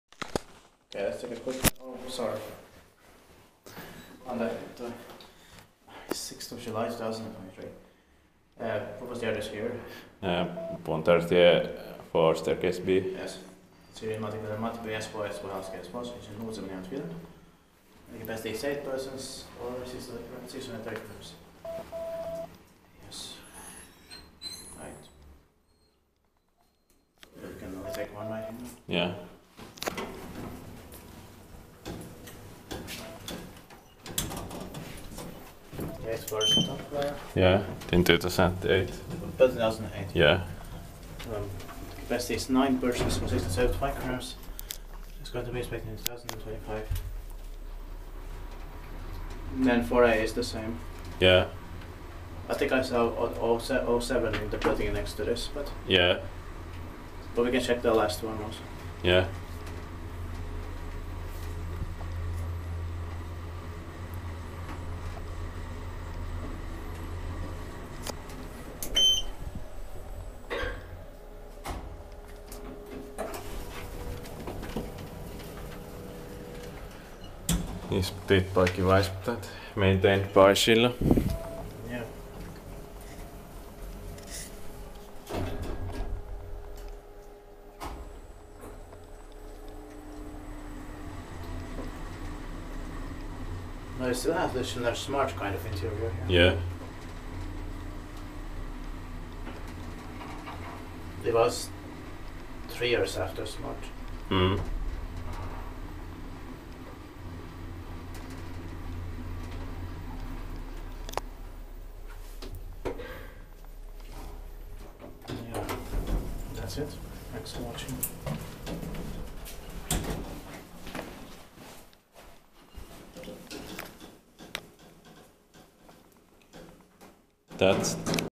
Schindler traction elevator/lift in Puntaritie 4B, Matinkylä, Espoo
Location: Puntaritie 4B, Matinkylä, Espoo, Finland
Brand: Schindler, Filmed with Elevators from Finland
Year installed/modernized: 2008
Serial number: Forgot to check
Floors: 8(1, 2, 3, 4, 5, 6, 7, 8)
Capacity: 8 persons or 630 kg
Filmed: 6.7.2023